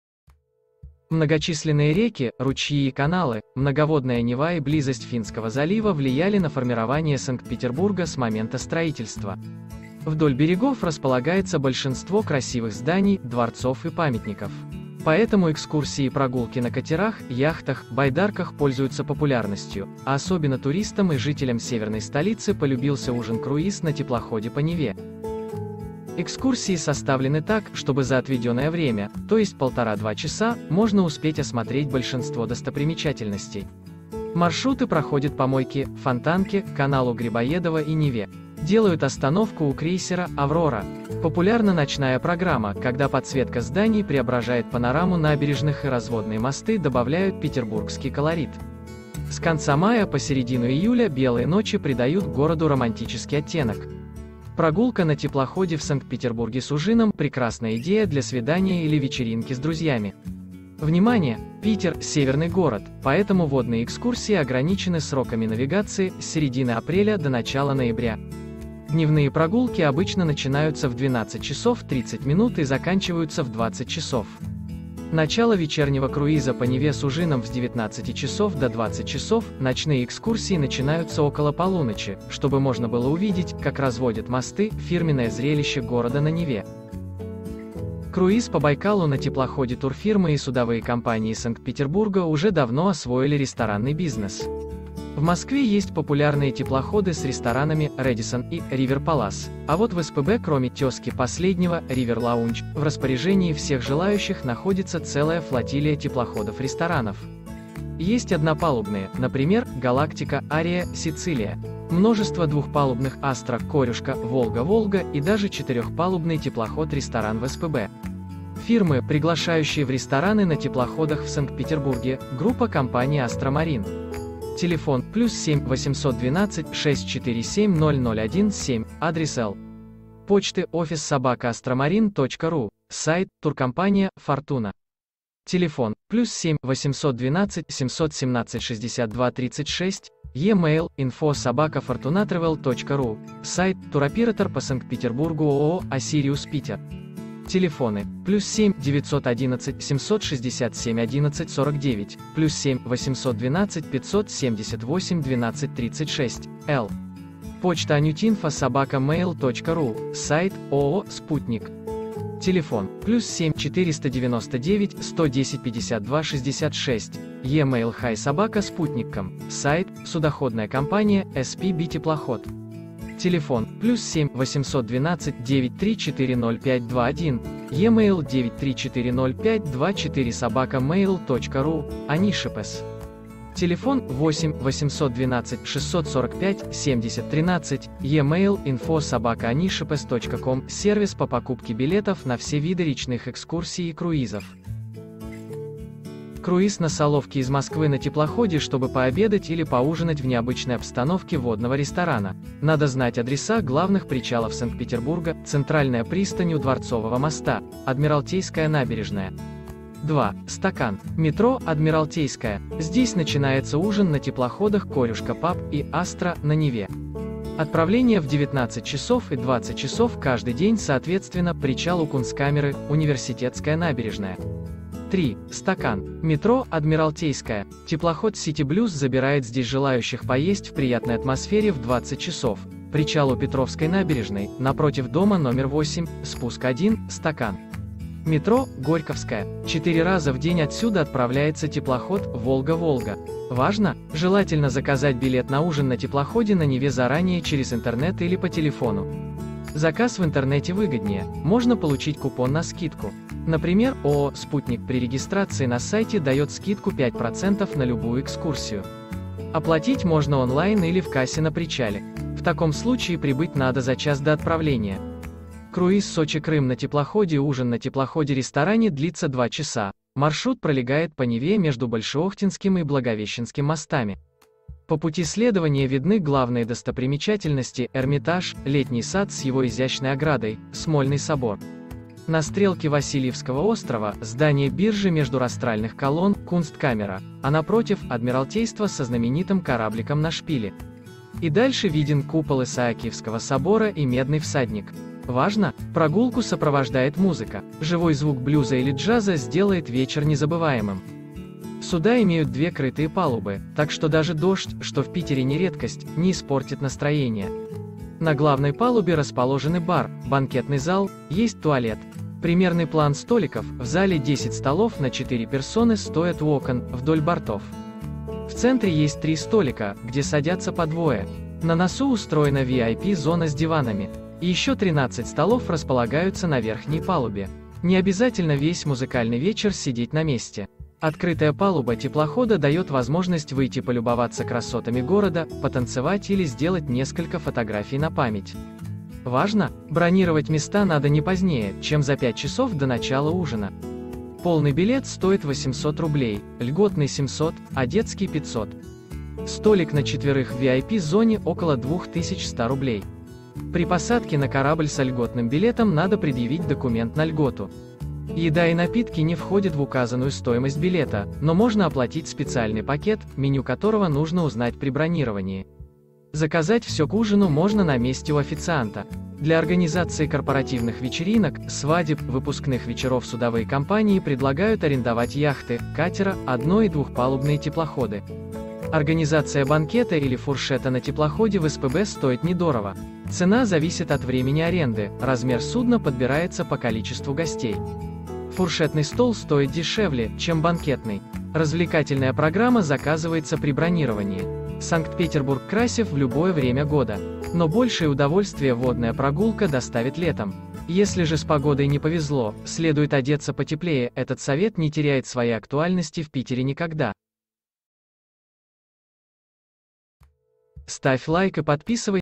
О круизах и прогулках на теплоходе в СПб с ужином: маршруты, рестораны
Круиз на теплоходе с ужином в СПб. Узнать расписание ресторанов-теплоходов в СПб. Маршруты по Неве, дневные прогулки, ночные дискотеки на теплоходах. Что входит в стоимость билета на теплоход. Дополнительный сервис для пассажиров.